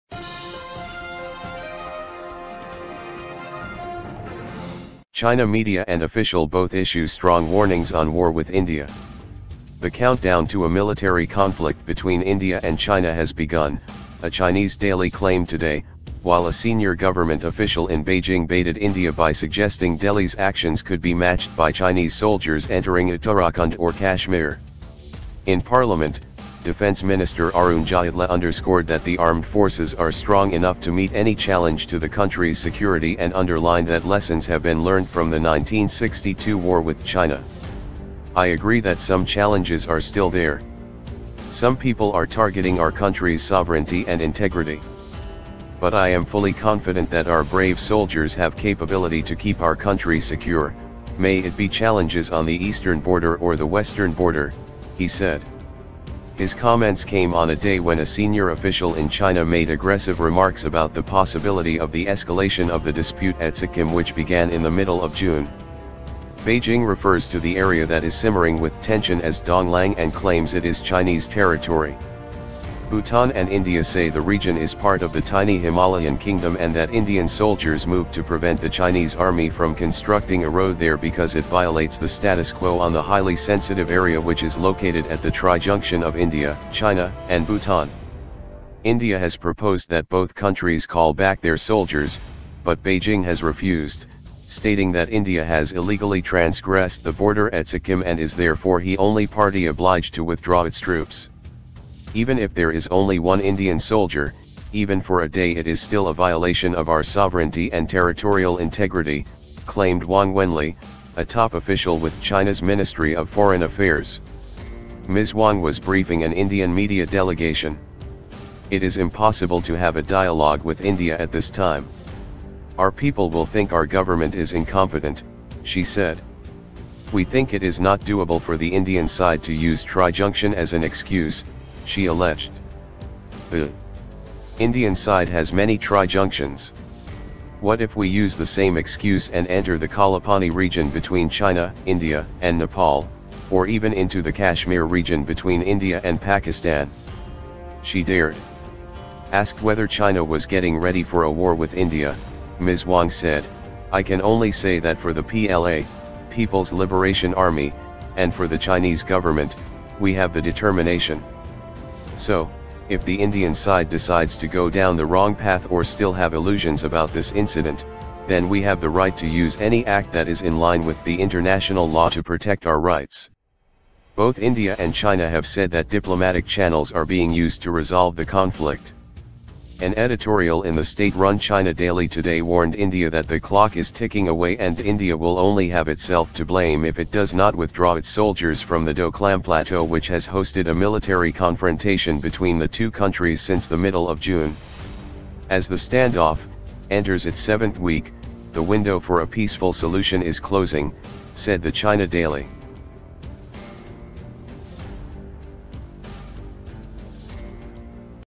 China Media And Official Both Issue Strong Warnings On War With India
The countdown to a military conflict between India and China has begun, a Chinese daily claimed today, while a senior government official in Beijing baited India by suggesting Delhi's actions could be matched by Chinese soldiers entering Uttarakhand or Kashmir.

In Parliament, Defence Minister Arun Jaitley underscored that the armed forces are strong enough to meet any challenge to the country's security and underlined that lessons have been learnt from the 1962 war with China. "I agree that some challenges are still there. Some people are targeting our country's sovereignty and integrity. But I am fully confident that our brave soldiers have capability to keep our country secure, may it be challenges on the eastern border or the western border," he said.